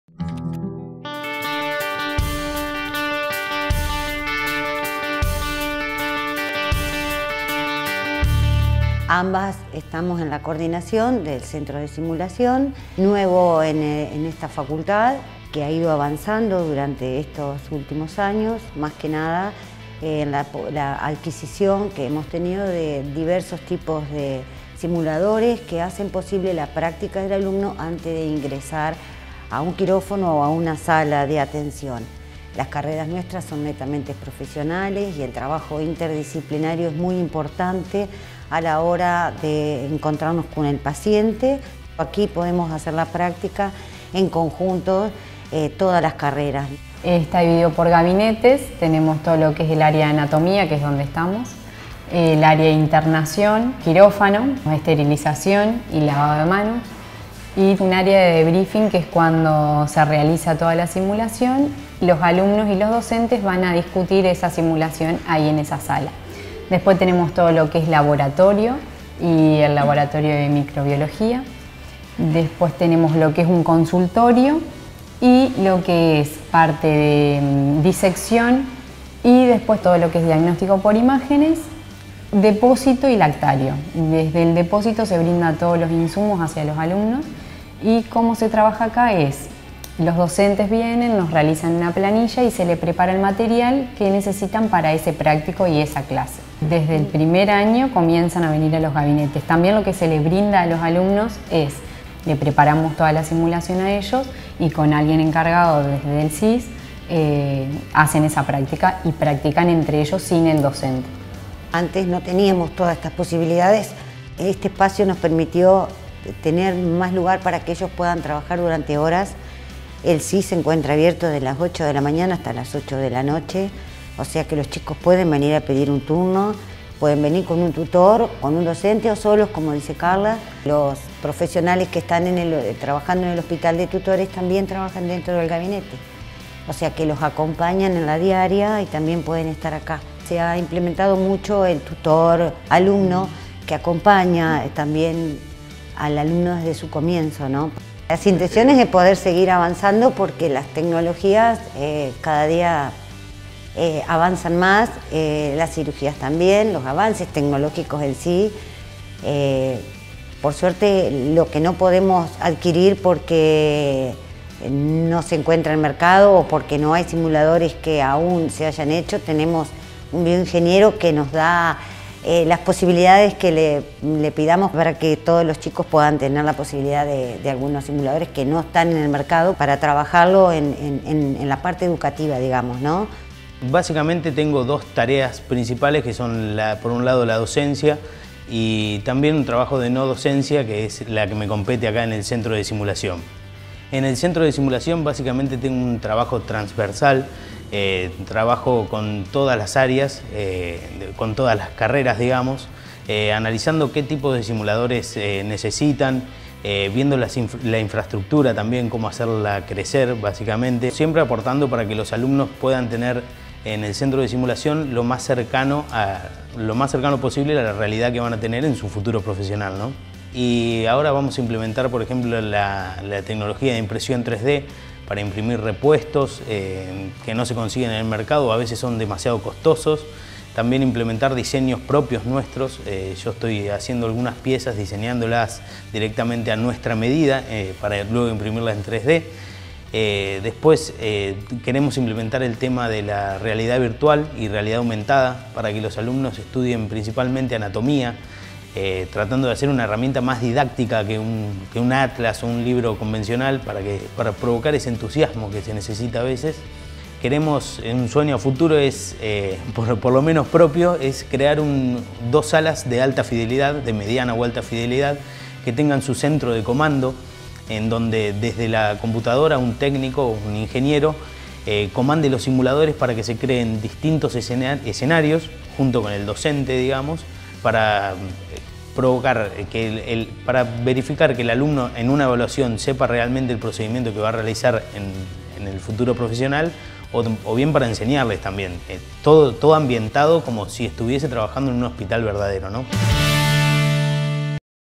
Simuladores en Salud
En la Facultad de Ciencias de la Salud de Concepción del Uruguay funciona el Centro Interdisciplinario de Simulación en Salud, donde los alumnos de las diferentes carreras pueden realizar sus prácticas como si estuviesen en un hospital real.